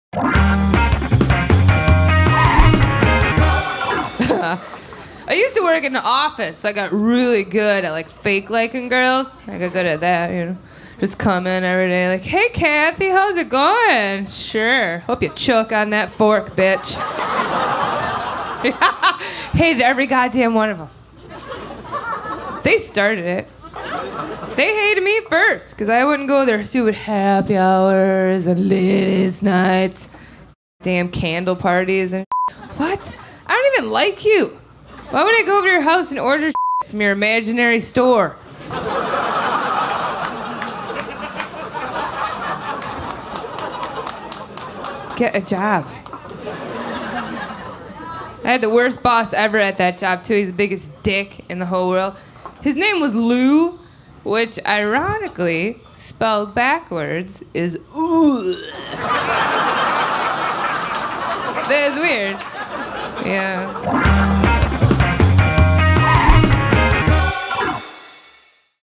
Don't Tell Anyone, But We Hate Our Coworkers Too
On the bright side, the worst offices make for the best comedy.